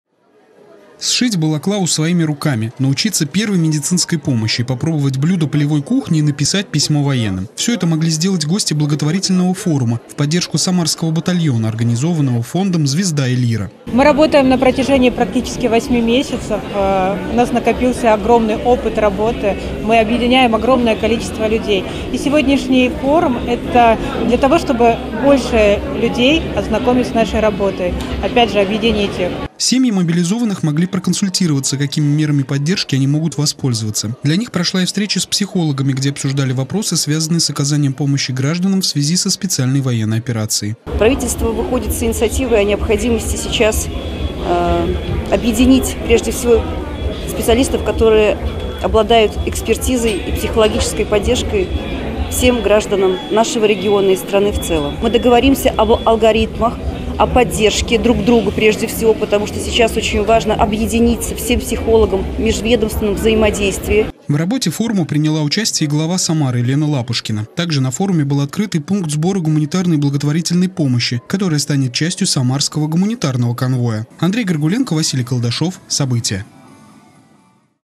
В Самаре прошел благотворительный форум от фонда "Звезда и лира"
Для поддержки военных, что защищают рубежи страны. В Самаре прошёл благотворительный форум, организованный общественным фондом "Звезда и лира". Различные тематические площадки были развёрнуты в Окружном доме офицеров. Гостем форума стала и глава Самары Елена Лапушкина. Подробнее в нашем следующем сюжете.